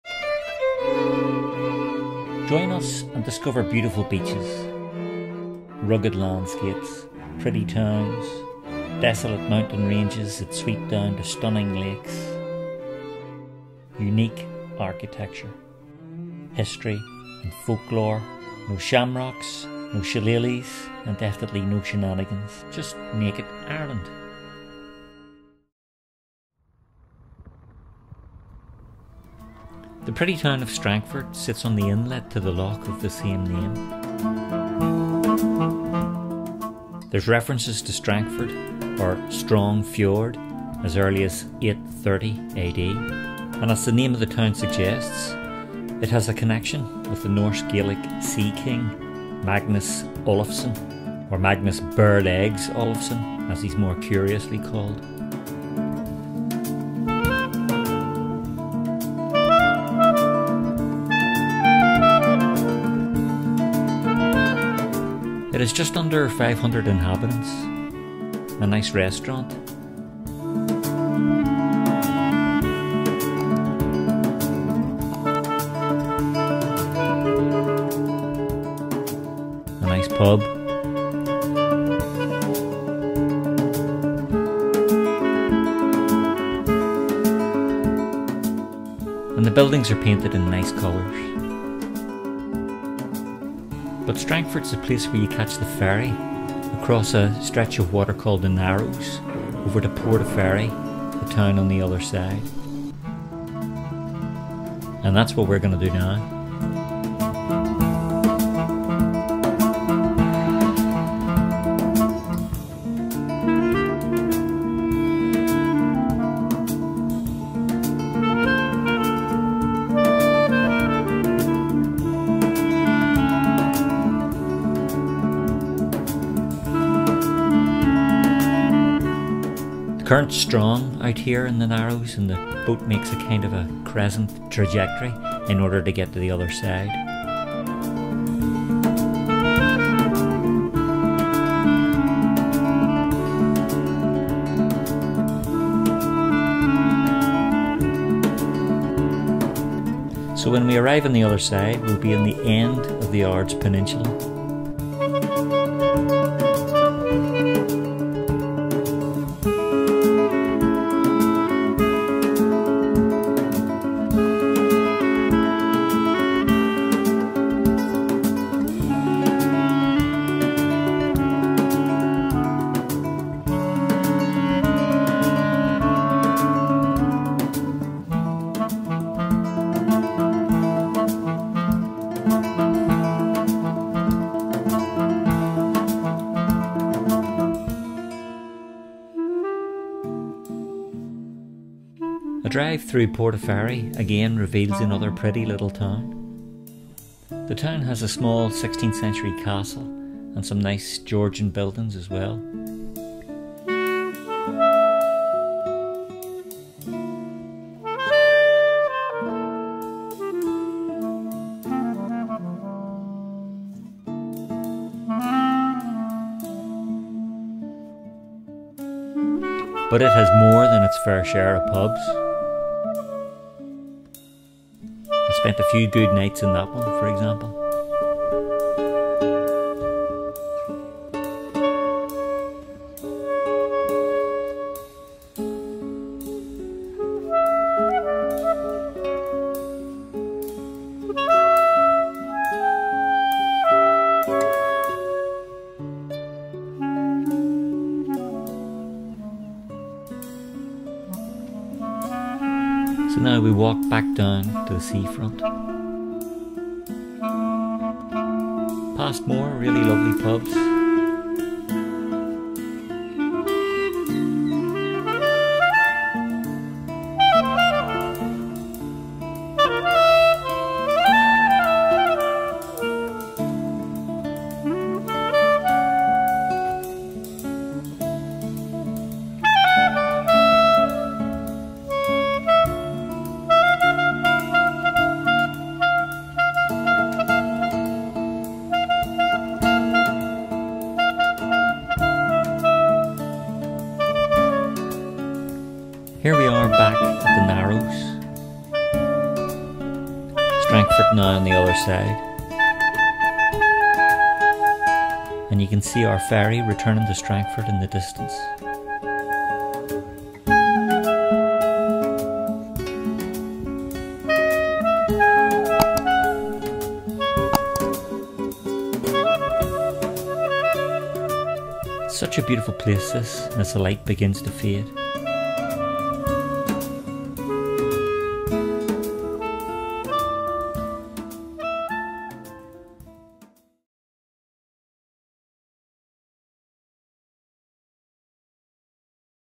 A visit to the village of Strangford and then a trip on the ferry across the narrows to Portaferry.
In this video we visit the town of Strangford in Northern Ireland, with its Norse roots (Strong fjord), home of King Magnus (bare legs) Olaffson. The name of the area is documented as early as c.830 and its Irish Name, Baile Loch Cuan means town on Strangford Lough. Then we take the ferry across the narrows of Strangford Lough to the town of Portaferry, which has a small castle, some nice Georgian buildings and quite a few lovely pubs. This is a nice introduction to County Down and to the Ards Peninsula area.